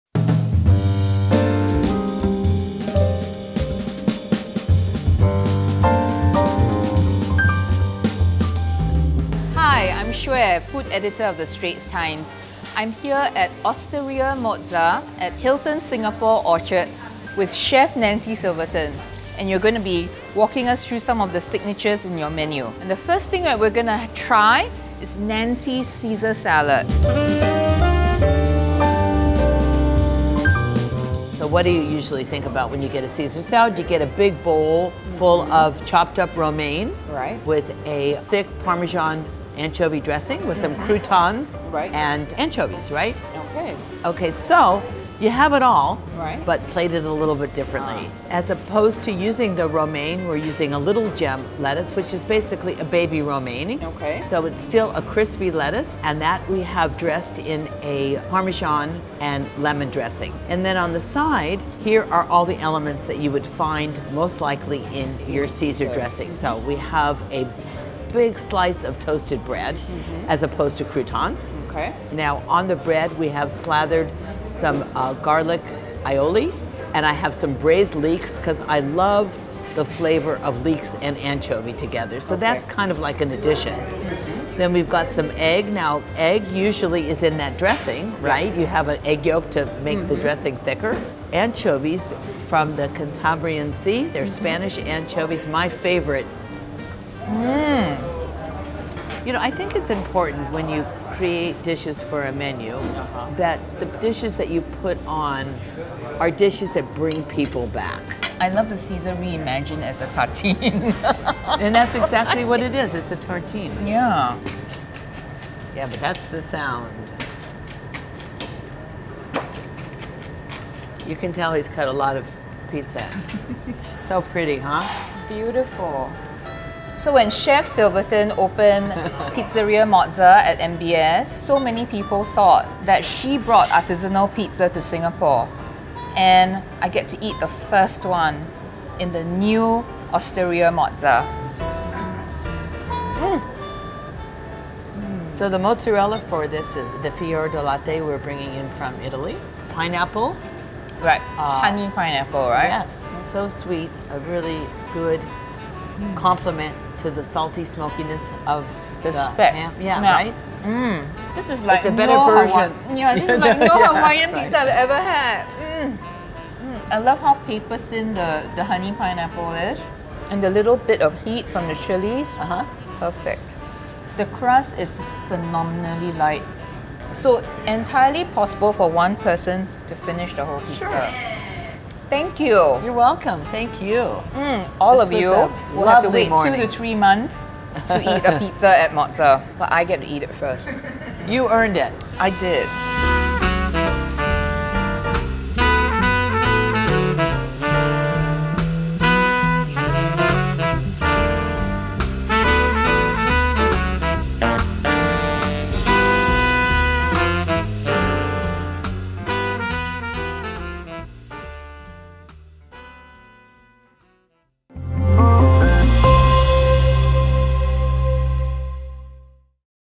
Osteria Mozza's Nancy Silverton and her famous pizzas
American chef Nancy Silverton is in Singapore to open Osteria Mozza at the Hilton Singapore Orchard. She shares her version of a classic Caesar salad and shows ST food correspondent Tan Hsueh Yun what goes into the making of her famous pizza.

American chef Nancy Silverton opens Osteria Mozza at Hilton Singapore Orchard